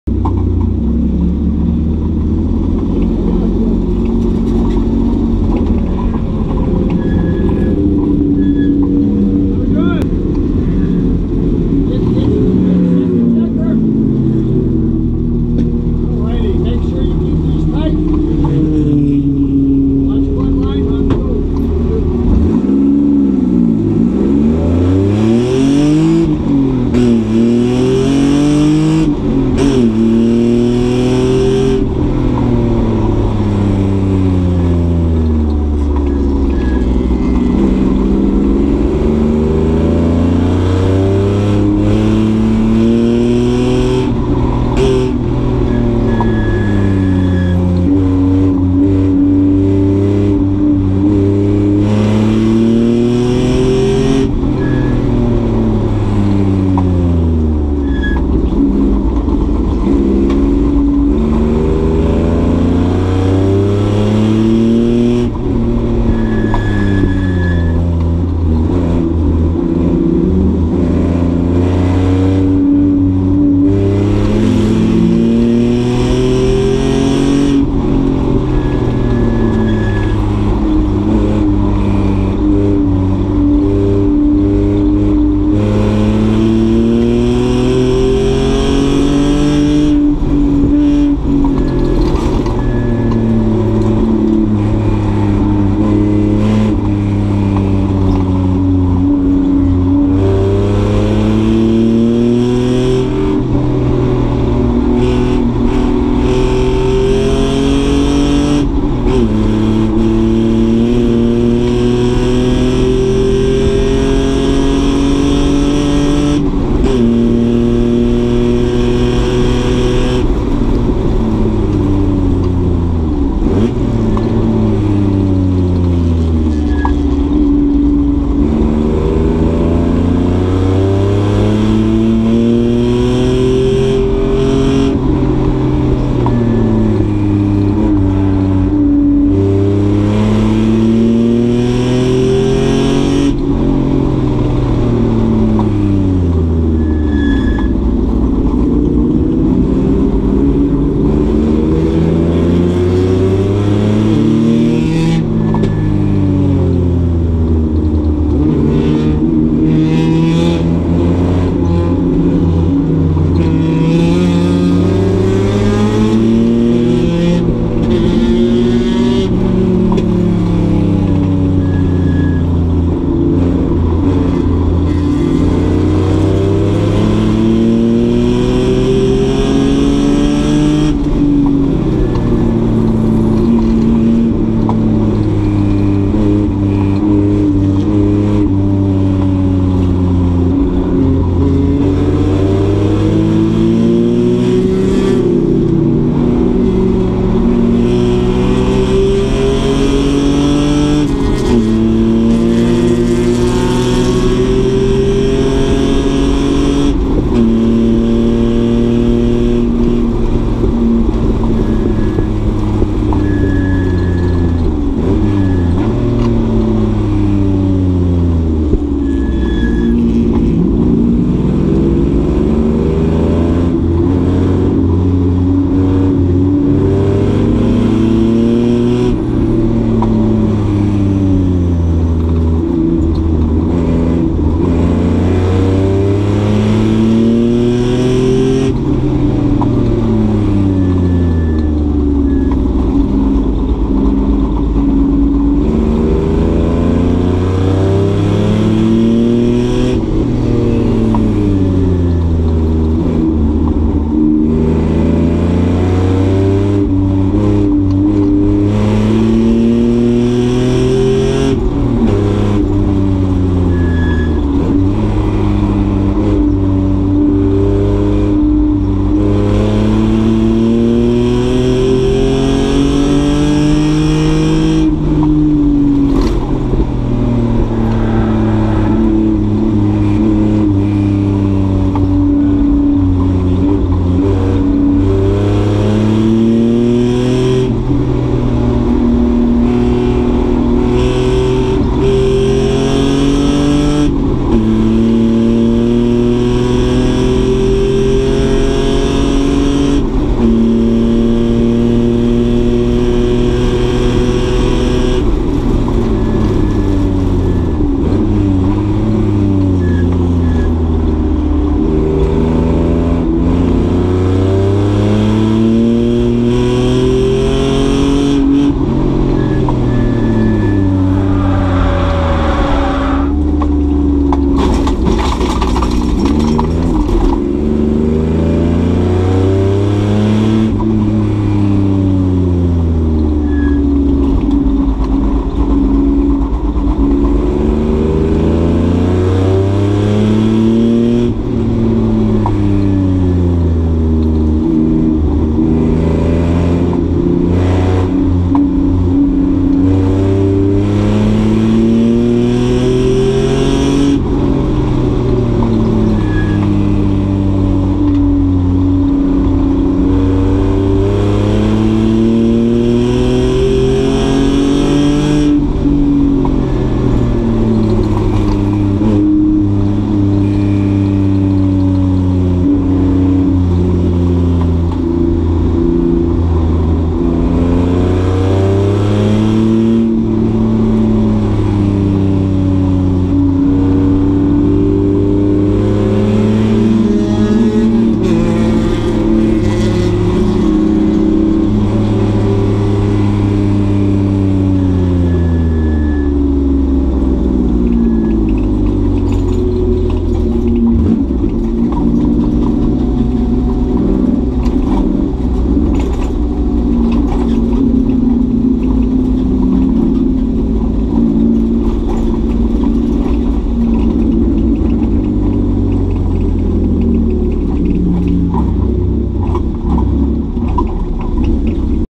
TSMP Day2/Shift5 Andy (24hoursoflemons)
Event: 24 Hours of LeMONS (August 2019)
Track: Thompson Speedway Motorsports Park
Driver: Andy
Description: Day2/Shift5